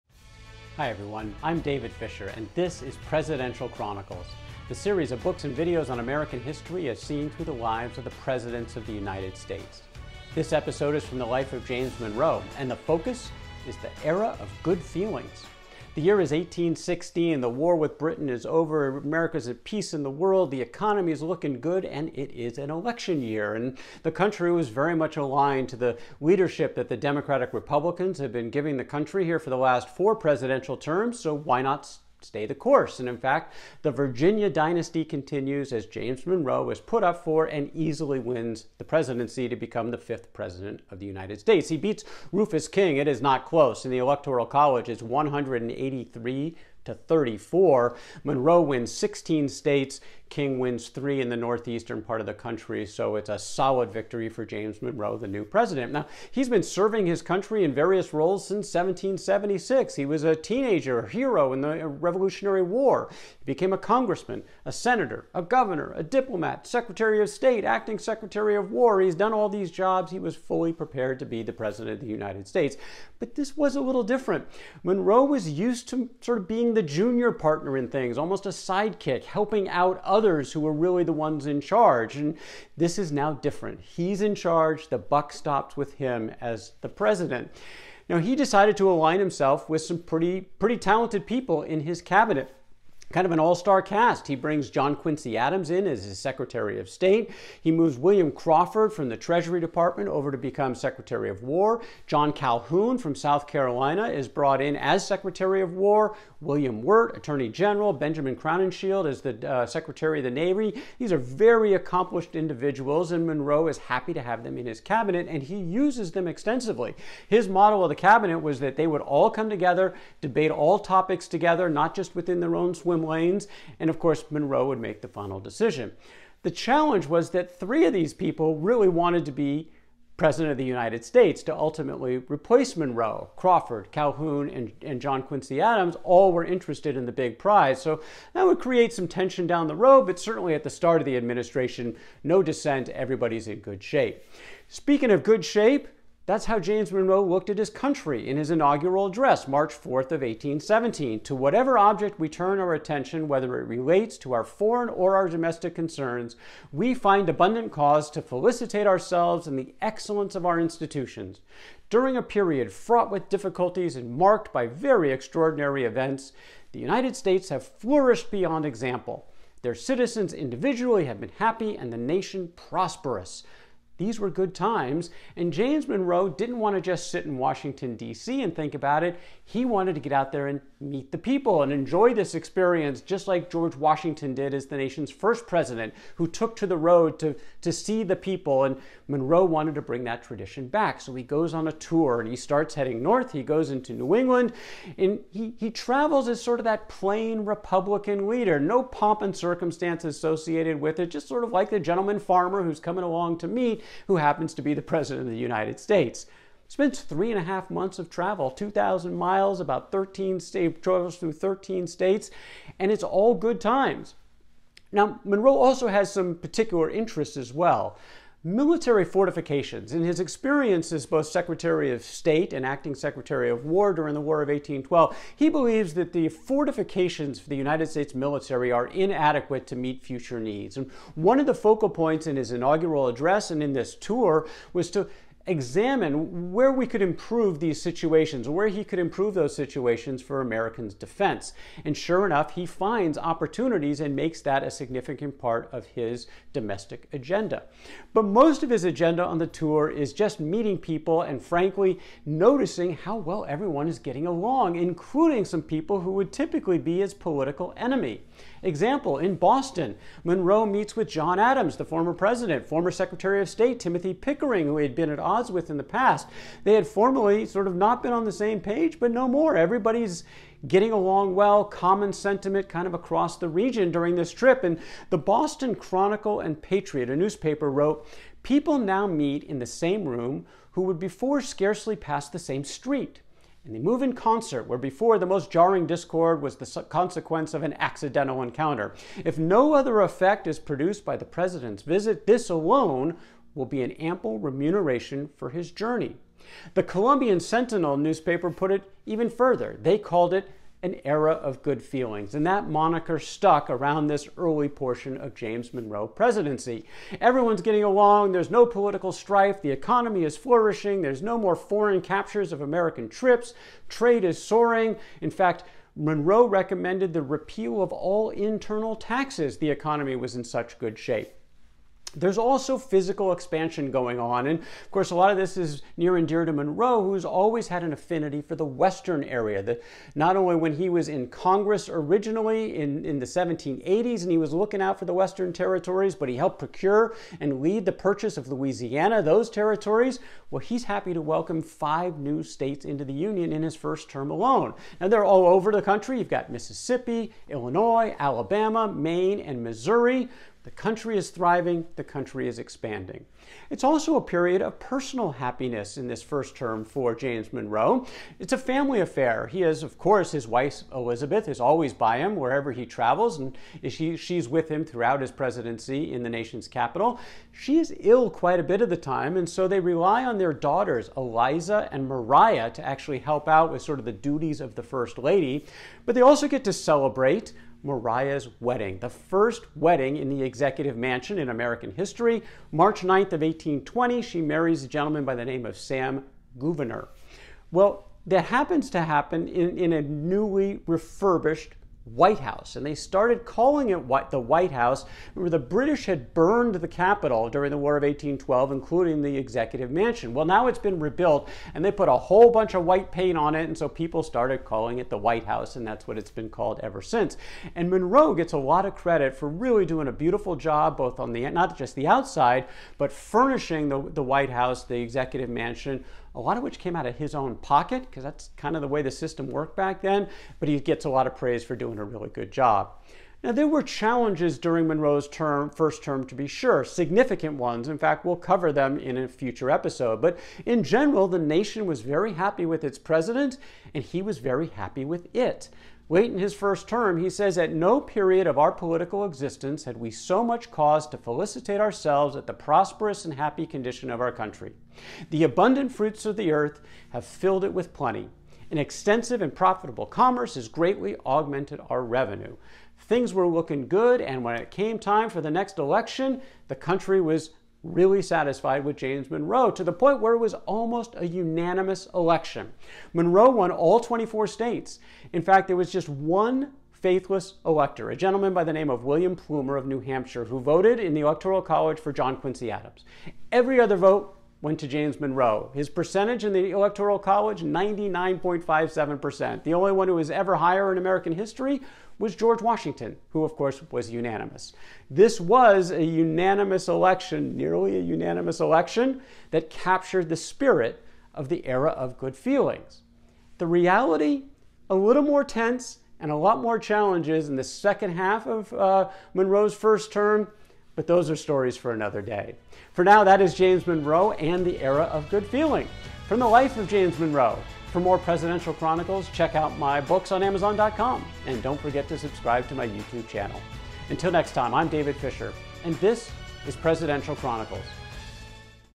James Monroe #6: Era of Good Feelings (1817-1820)
Series: Presidential Chronicles; Episode: James Monroe #6: Era of Good Feelings (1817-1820)

Topics covered include: Elected President of the United States, Virginia Dynasty, Cabinet, tour of New England, Era of Good Feelings, economy flourishing, reduced partisan political strife, improved foreign relations, family in the White House, wedding for daughter Maria (first WH wedding), and nearly unanimously re-elected

Key people: Rufus King, John Quincy Adams, William Crawford, John Calhoun, William Wirt, Benjamin Crowninshield, John Adams, Timothy Pickering, Elizabeth Monroe, Eliza Monroe, Maria Monroe, Samuel Gouverneur, and William Plumer